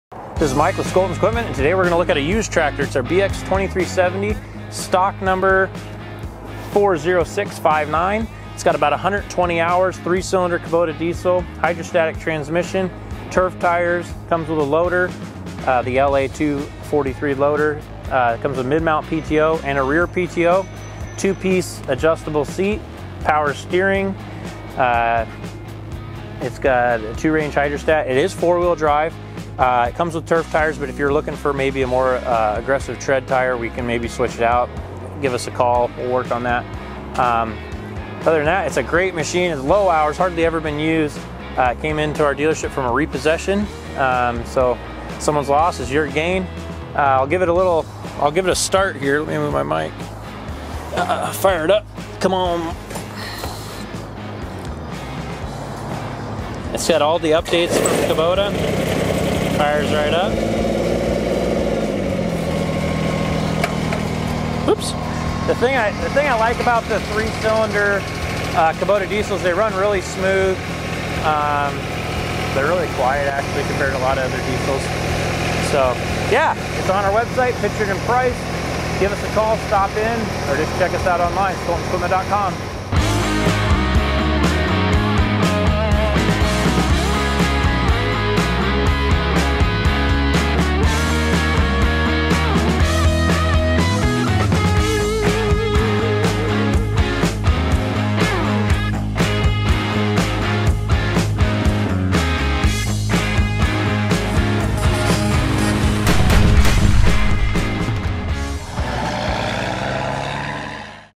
Used Kubota BX2370
Here's a quick look and walk around of our used Kubota BX2370D with 113 Engine house and 23 Horse Power this tractor is sure to sell fast.